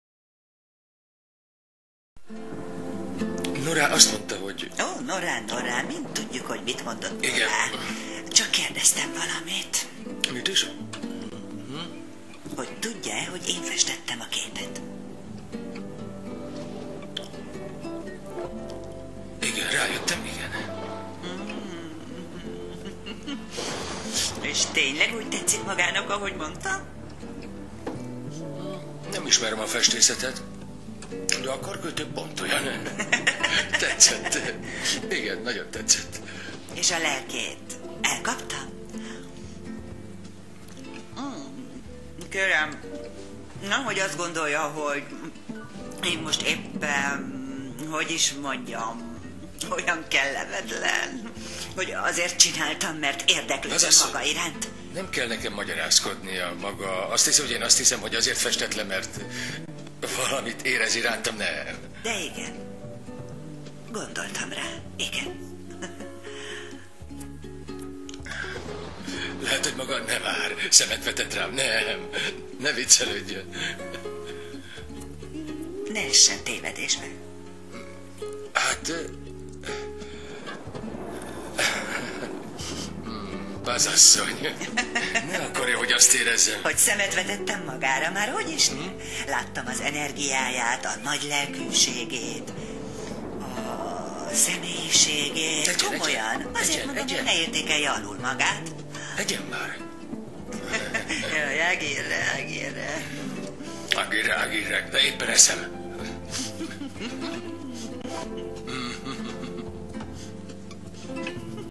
Páz asszony és Ágirre_Ágirre zavarba kerül
Páz asszony elkezd beszélni a képről, ekkor Ágirre teljesen zvarba kerül, elpirul:D ...